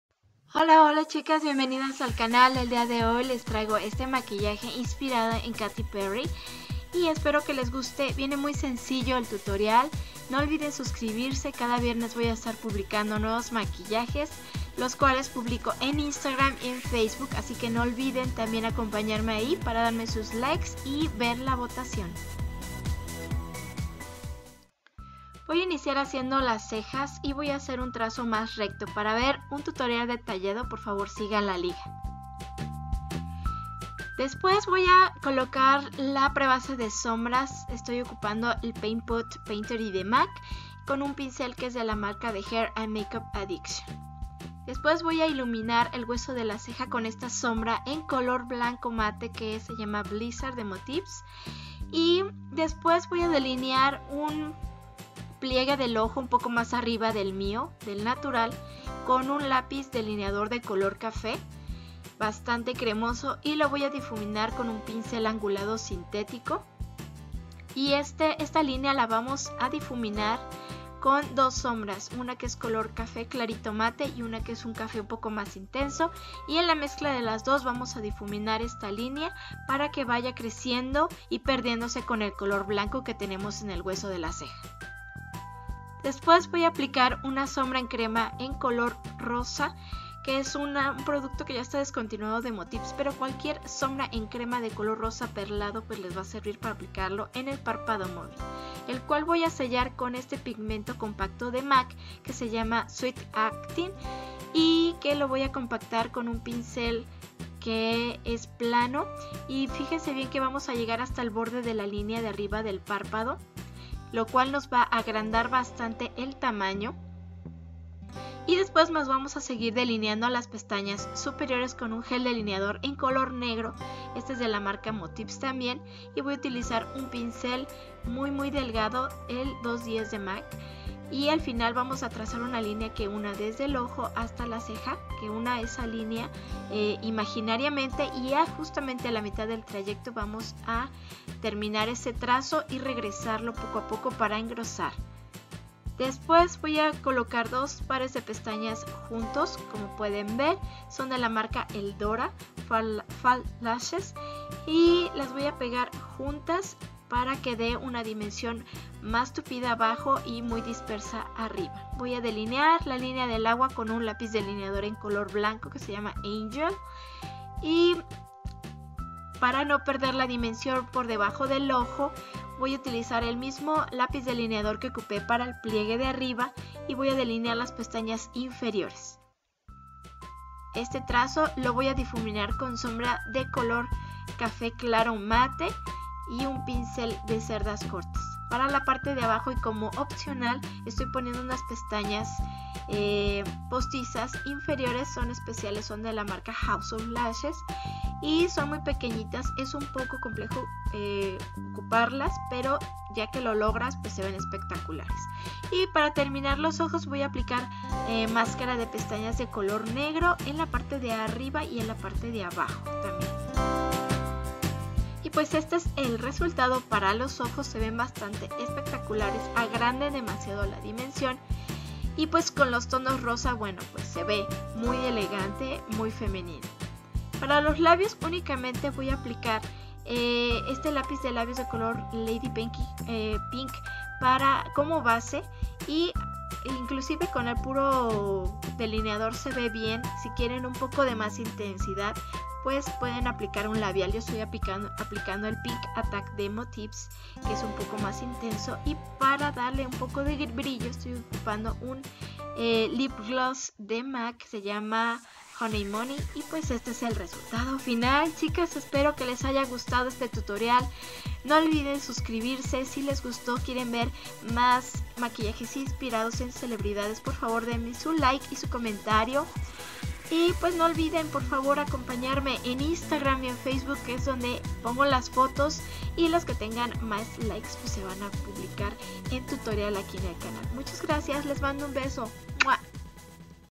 Maquillaje KATY PERRY /MAKEUP ( link to English version)
En este video encontraras los pasos para lograr  un maquillaje femenino y dramatico como lo luce KATY PERRY, super sencillo y  detallado en cada producto usado. No olvides subscribirte al canal para que no te pierdas ningun video

Details/ Details:

BROWS/ CEJAS :
Dip Brow Pomade color EBONY de Anastasia Beverly Hills

EYES/OJOS:
Paint Pot Painterly  by @maccosmetics / prebase de sombras PAINTERLY de MAC

Pressed eye Shadows in CAPUCCINO & HOT CHOCOLATE by @motivescosmetics on the crease and below lower lashline / Sombra CAPPUCCINO y HOT CHOCOLATE de Motives by Loren Ridinger en le pliegue del ojo y por debajo de las pestanas inferiores

Cream eye shadow in BUBBLE GUM by @motivescosmetics ( discontinued) on mobile eyelid / Sombra en cream color rosa BUBBLE GUM  de Motives Cosmetics en el parpado movil ( esta descontinuado este producto)

Pressed Pigment in SWEET ACTING by @maccosmetics on mobile eyelid/ Pigmento comprimido SWEET ACTING de MAC Cosmetics

Gel eyeliner in LITTLE BLACK DRESS by @motivescosmetics on top lashline / Gel delineador negro LITTLE BLACK DRESS de Motives cosmetics en pestanas superiores

Pencil eyeliner in ANGEL by @motivescosmetics on waterline / lapiz delineador blanco ANGEL de Motives Cosmetics en la linea del agua

LASHES/ PESTANAS:
H154 & H159 by @eldorafalseeyelashes on top / Son de las H159 y H154 juntas arriba

Precious by @houseoflashes on bottom / abajo traigo las Precious de House of Lashes

LIPS/LABIOS :
Lip crayon in LADY PINK  and Lipstick PINK ATTACK by Motivescosmetics , plus DAZZESGLASS in HONEY, MONEY by MAC
Lapiz delineador de labios en LADY PINK y labial PINK ATTACK de Motives Cosmetics , ademas el brillo HONEY, MONEY de mac

Gracias!!! thank u!!